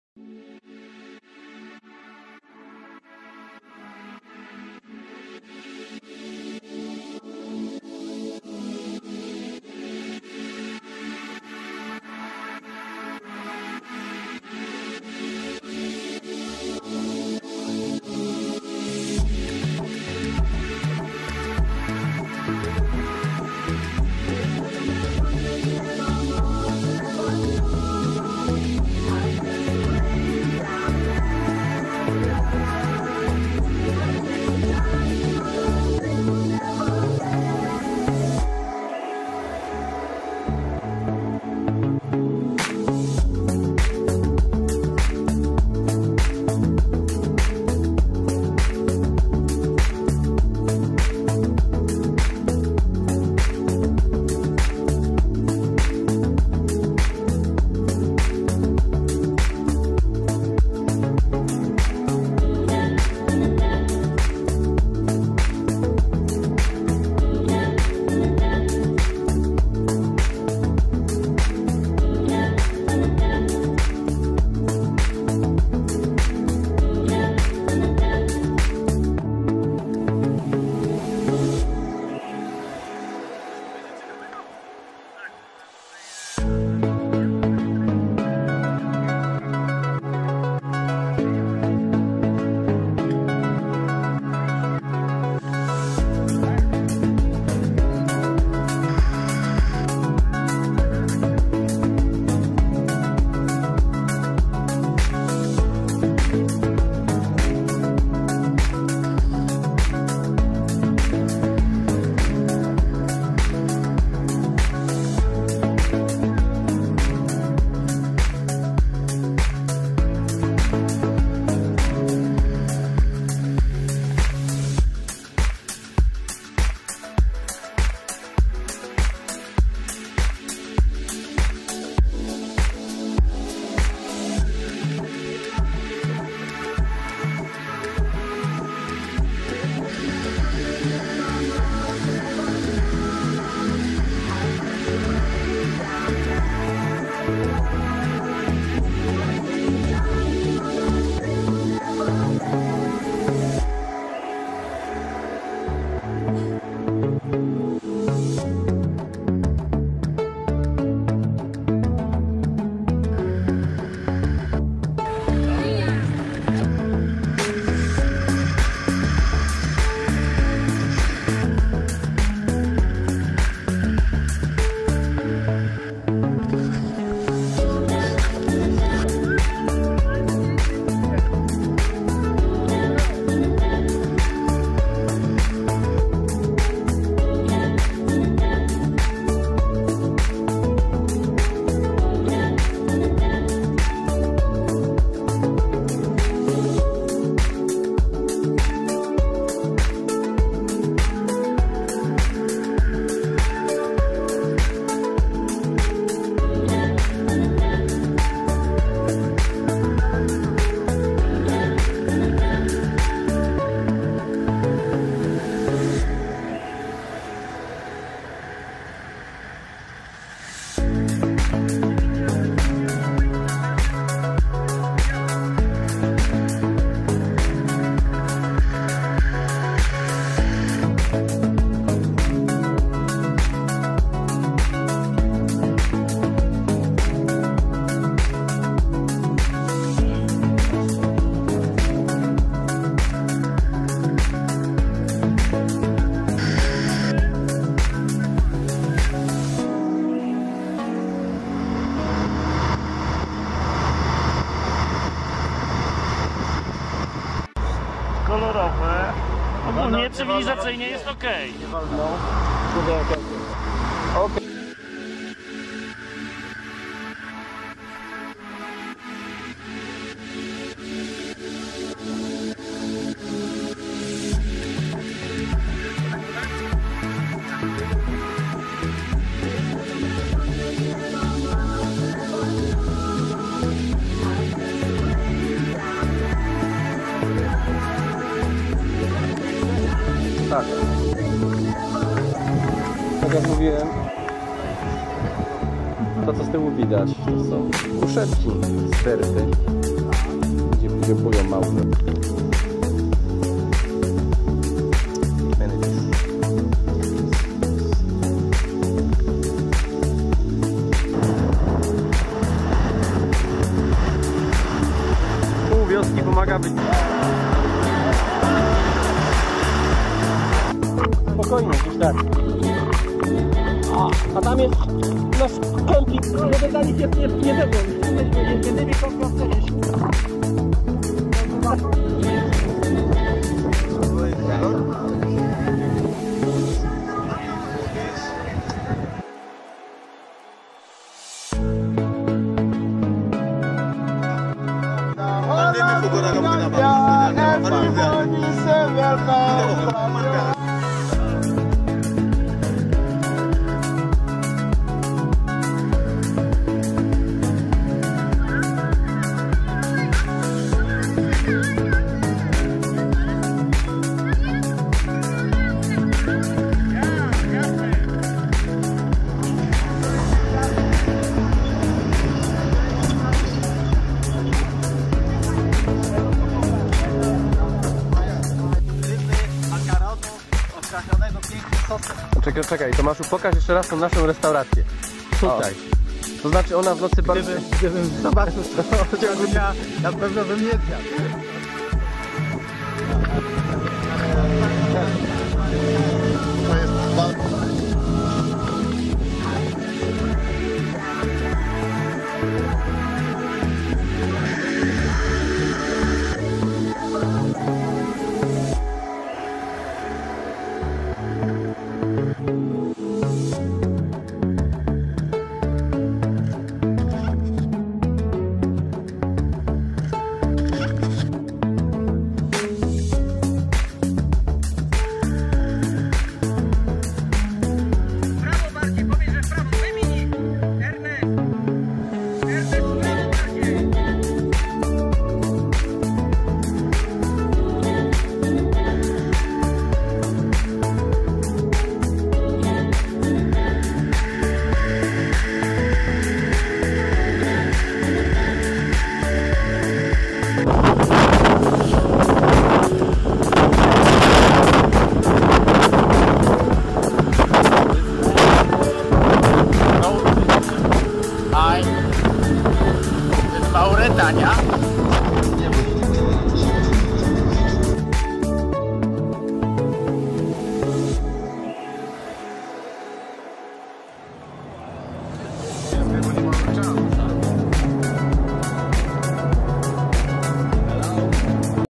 AFRYKA 2015 - Mauretania, Senegal, Gambia na motocyklu.
Wyjazd na motocyklu, Felix, Tomek, Neno, Janusz.
Kwiecień 2015.